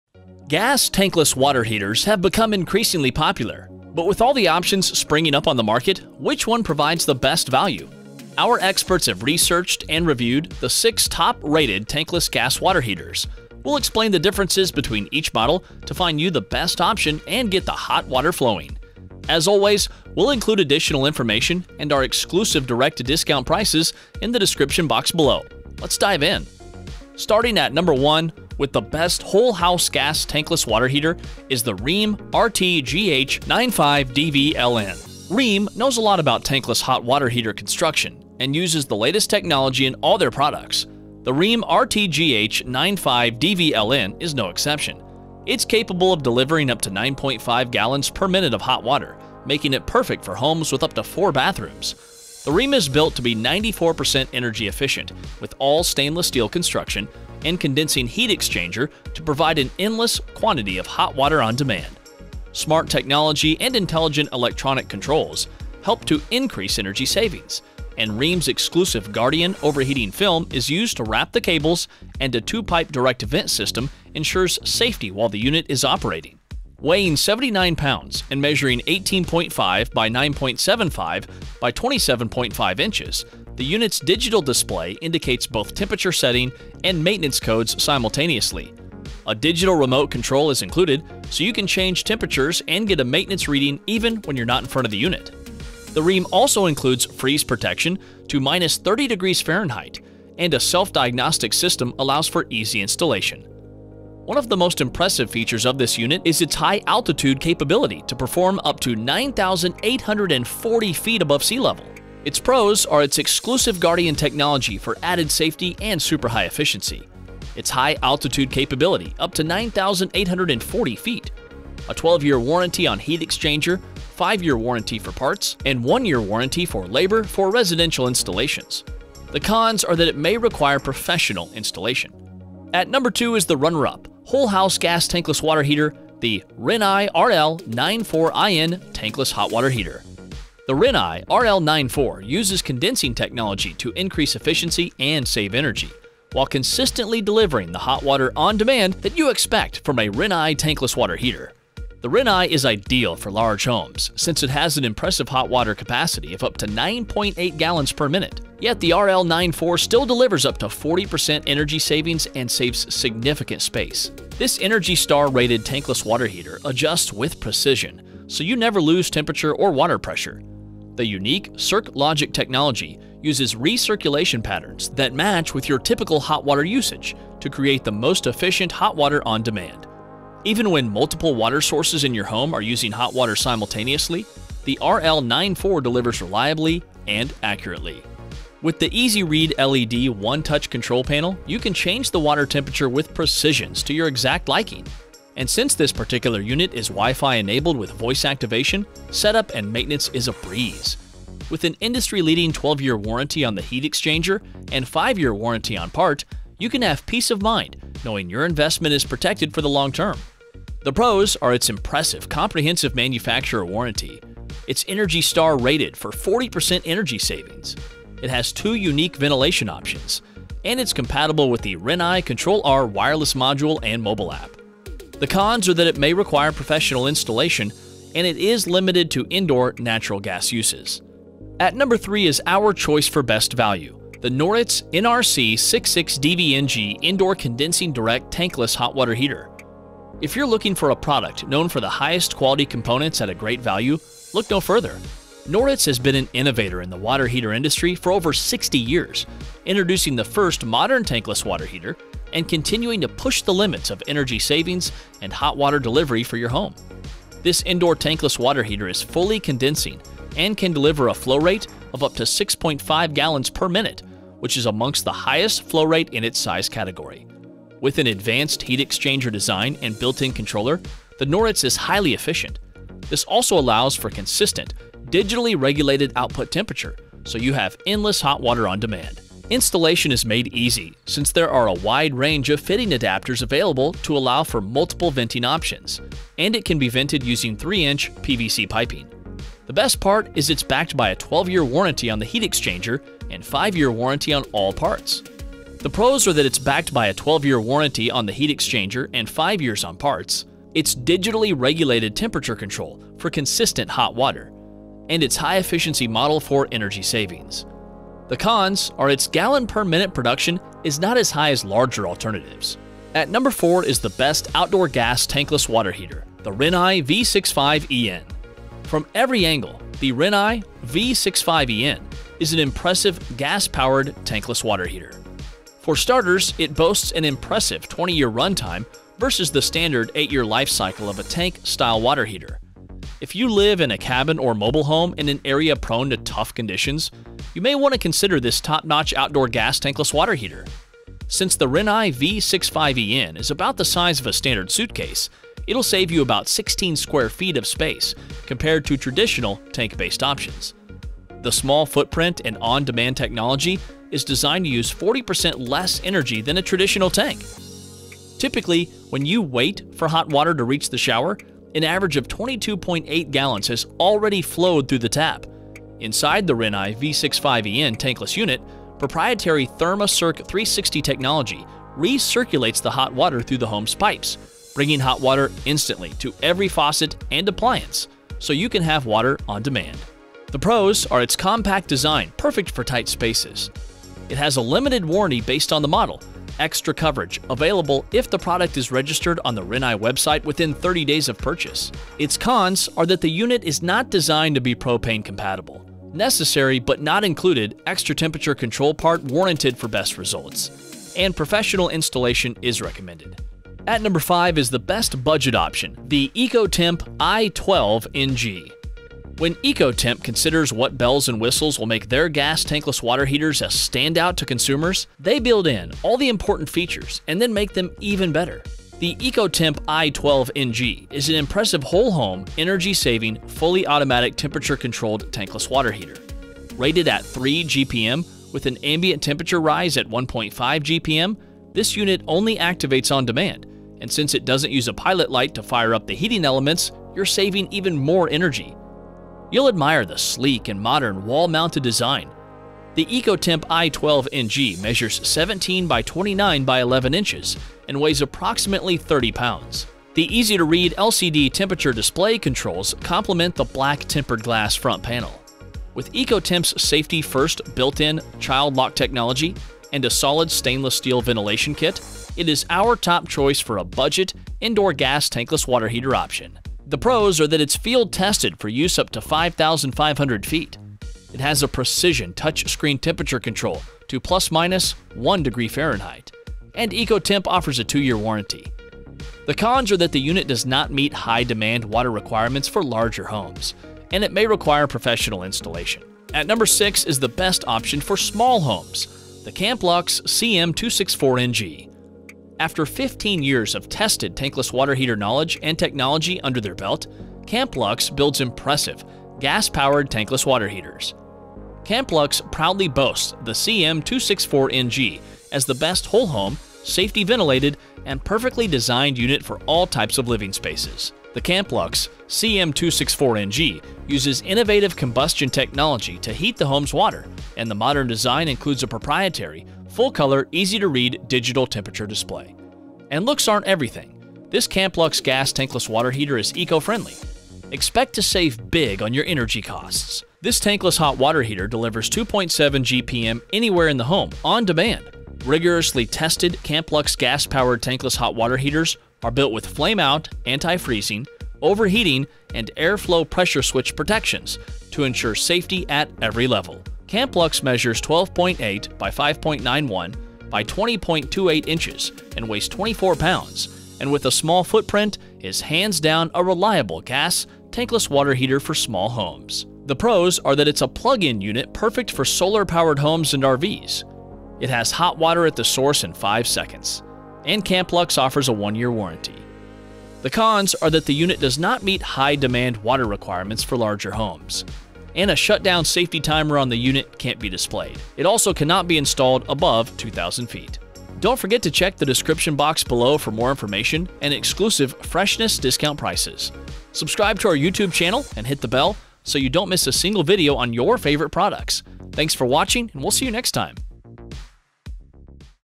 BEST Tankless Gas Water Heater Reviews ♨️ (Ultimate 2023 Natural Gas and Propane Water Heater Guide)
👇 Best Prices On Gas Tankless Water Heaters👇 0:00
1️⃣ Best Whole House Gas Tankless System: Rheem RTGH-95DVLN Gas Tankless Water Heater 0:31

2️⃣ Whole House System Runner Up: Rinnai RL94IN Tankless Gas Water Heater 2:14

3️⃣ Best Value: Noritz NRC66DVNG Indoor Condensing Direct Tankless Hot Water Heater 3:53

4️⃣ Best Outdoor Option: Rinnai V65eN Gas Tankless Water Heater 5:16

5️⃣ Best Budget Option: Eccotemp i12-NG Tankless Gas Water Heater 6:44

6️⃣ Best For RVs and Small Homes: Camplux CM264NG Gas Tankless Water Heater 8:06

💧🧪 Get your own water test kit and find out what may be lurking in your home's water!

Gas tankless water heaters have become increasingly popular, but with all the options springing up on the market, which one provides the best value? Whether with propane or natural gas, we'll look at gas water heater for the whole house, RVs, and indoor/outdoor models.

Our experts have researched and reviewed the six top-rated tankless gas water heaters. We’ll explain the differences between each model to find you the best option and get the hot water flowing.

Don't forget to check the links above for exclusive factory-direct discounts!